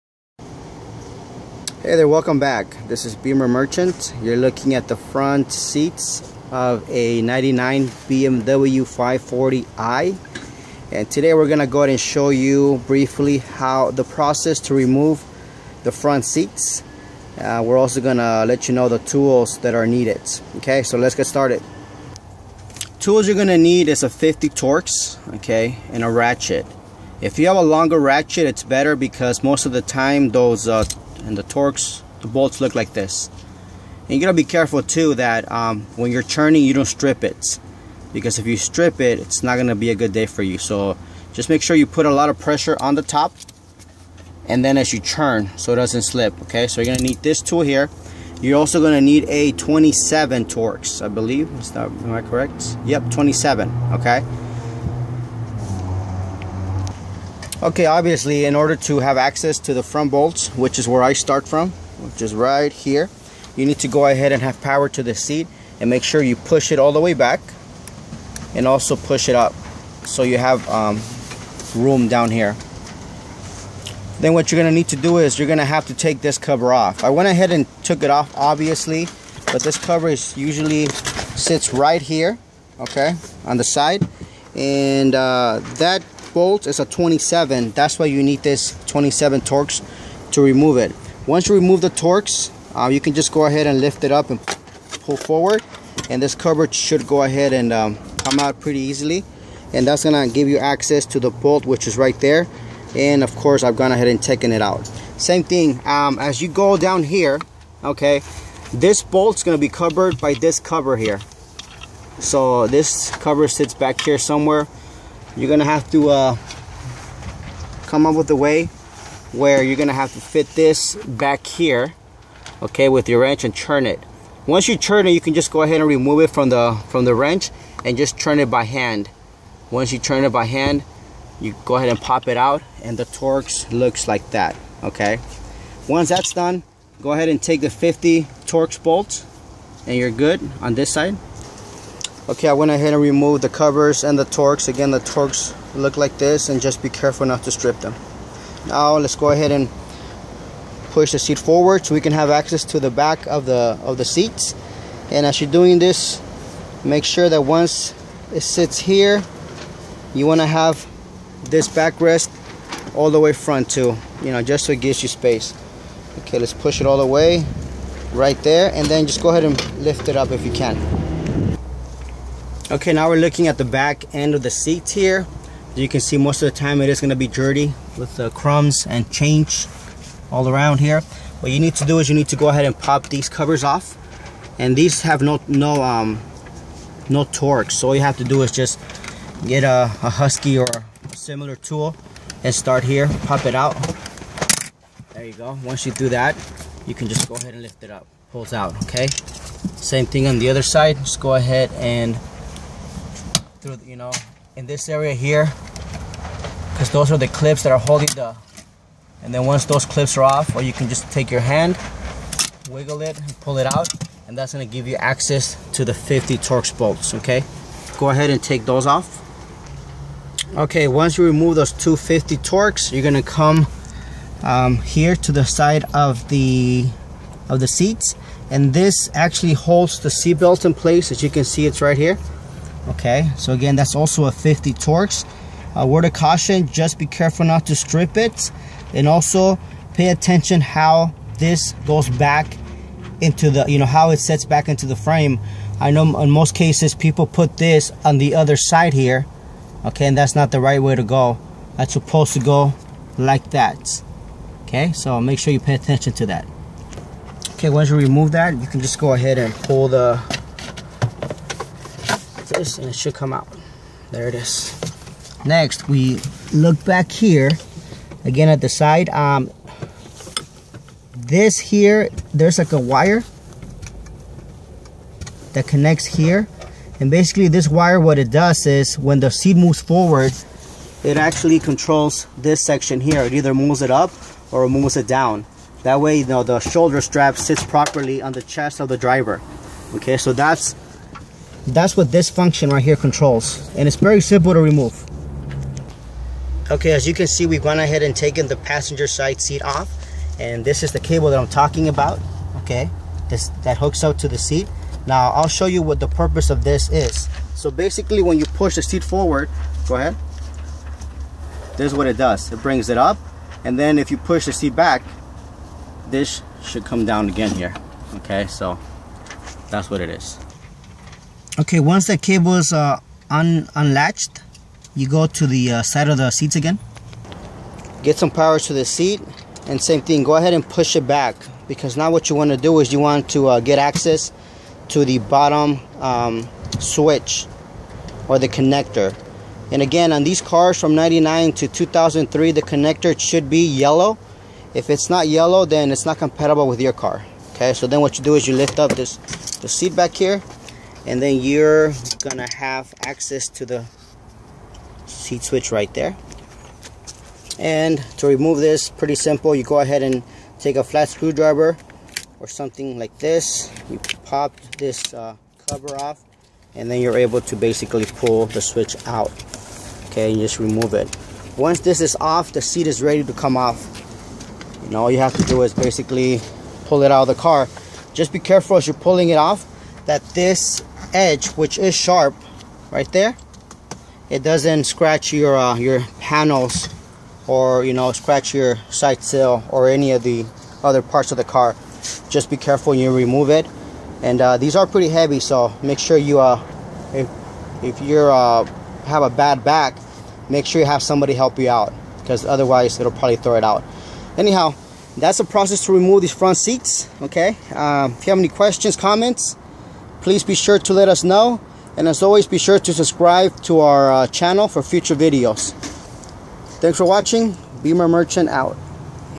How to remove front electric seats on a Bmw E39 540i 525i 530i and BMW 740i
Time to replace those old beat up front seats from your bmw 5-series?  Well, you have come to the right place.  This video will show you how to remove the front seats and what tools you'll be needing.  Once your are done, your interior should look much better.

Check us out for used BMW oem parts !